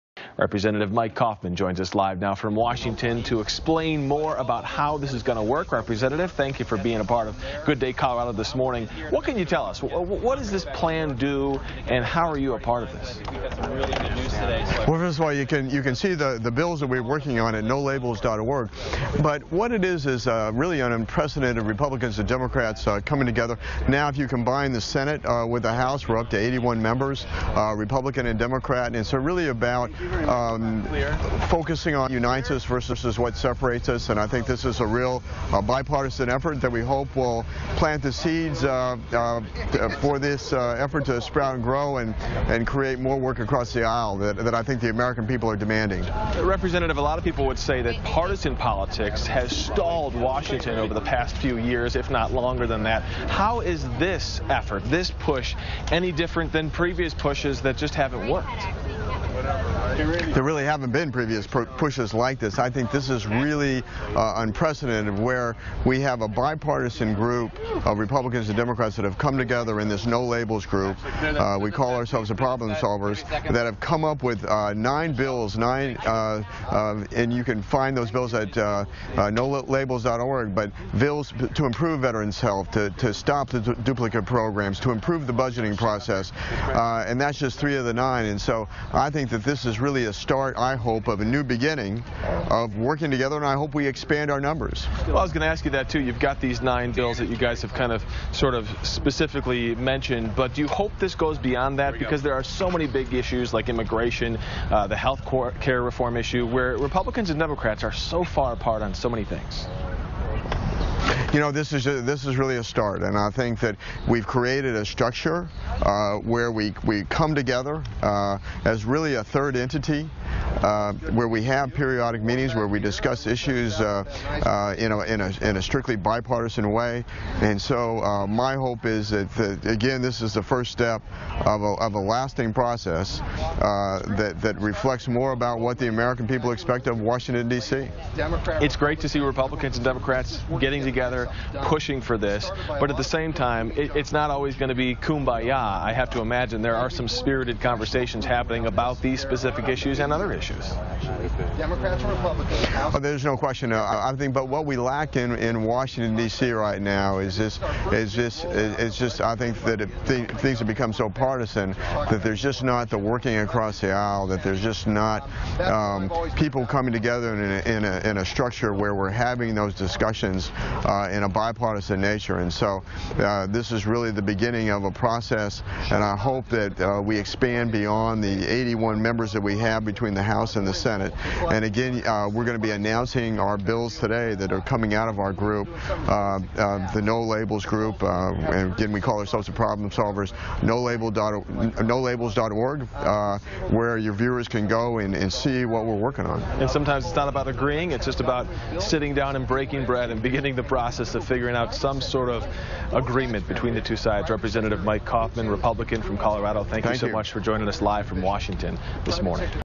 Rep. Mike Coffman Speaks about No Labels
Rep. Mike Coffman talks about the work of the Problem Solvers on the "Make Government Work!" legislative package. For more information about No Labels, the Problem Solvers and Make Government Work!,